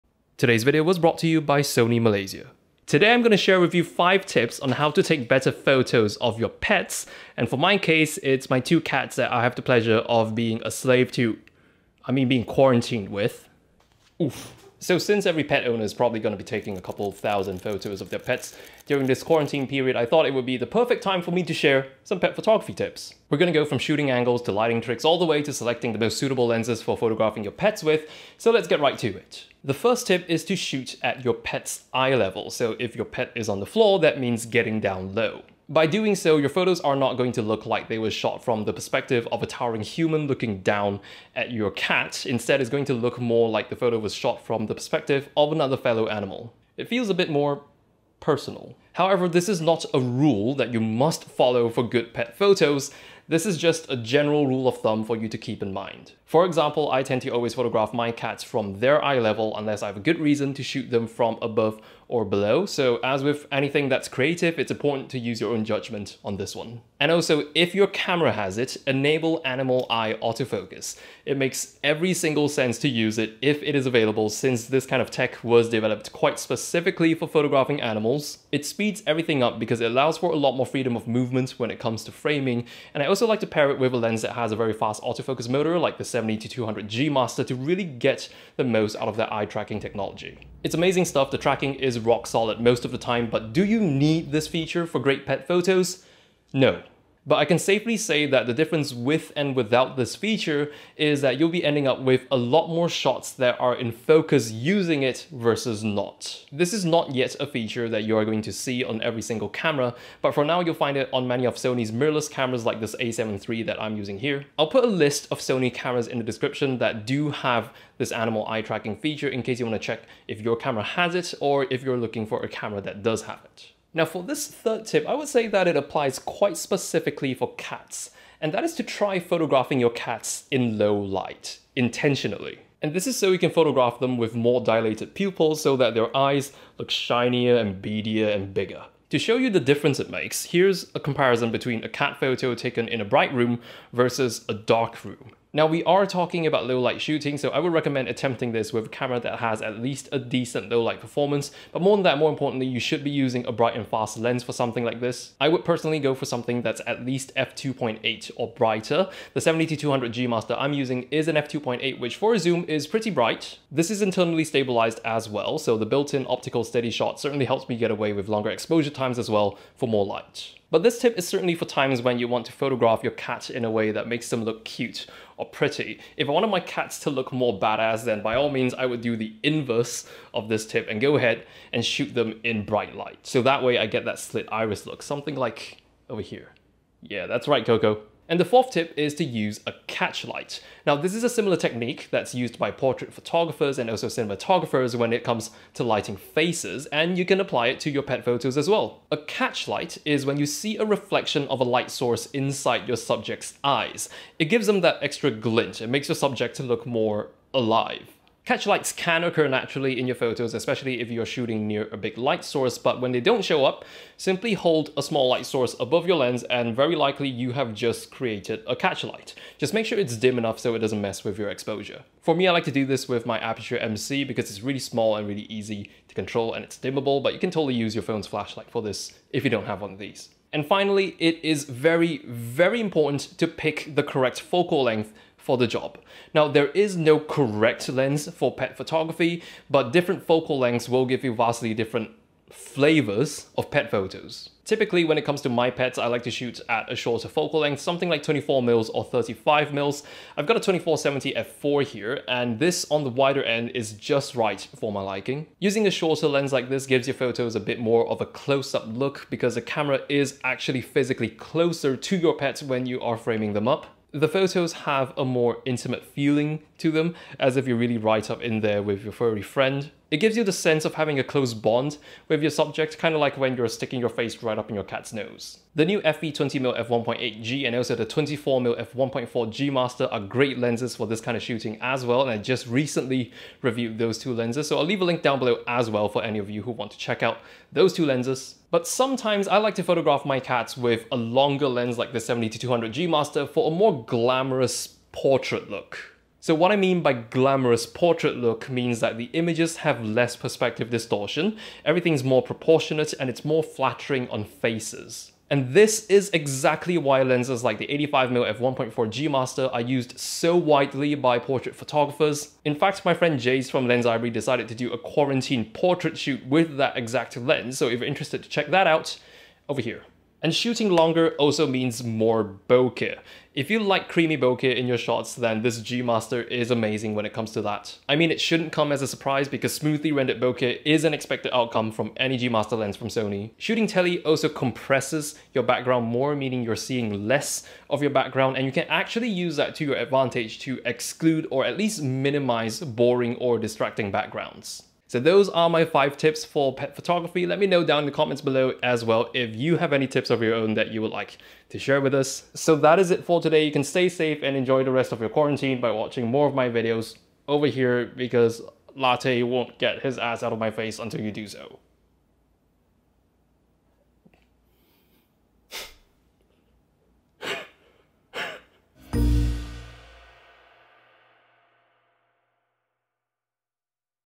How To Take Better Pet Photos - 5 Simple Tips
StayHome Pet Photography Tips featuring the Sony FE 70-200 F2.8 GM OSS!

Here's a list of Sony cameras with Animal Eye AF (as of April 2020)
a9 II, a6600, a6100, a9, a7R IV, a7R III, a7 III, a6400, RX100 VII, RX10 IV

You can also license stock footage from Artgrid.io (same 2 bonus months applies!):